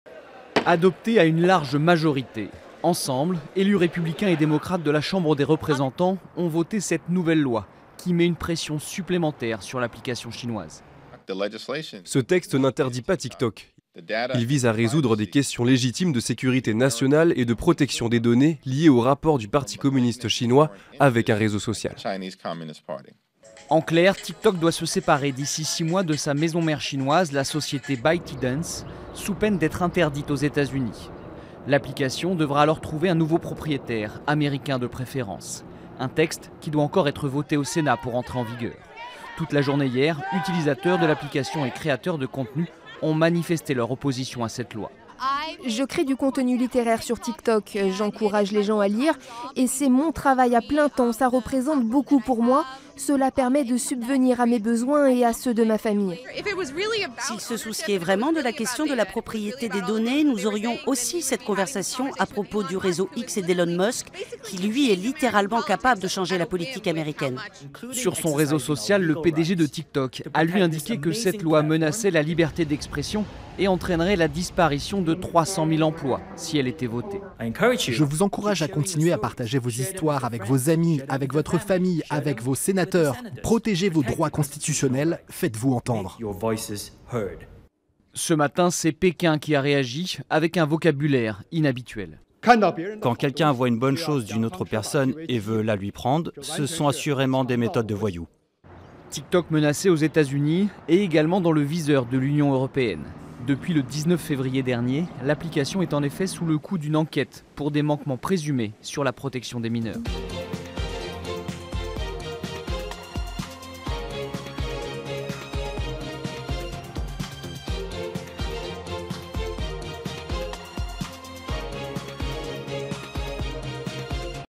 TikTok bientôt interdit aux Etats-Unis ?
Accusé d’espionner et de manipuler ses 170 millions d’utilisateurs américains, TikTok pourrait être banni aux États-Unis. La Chambre des représentants a adopté une proposition de loi visant l’interdiction du réseau social, à moins qu’il se sépare de sa maison mère ByteDance.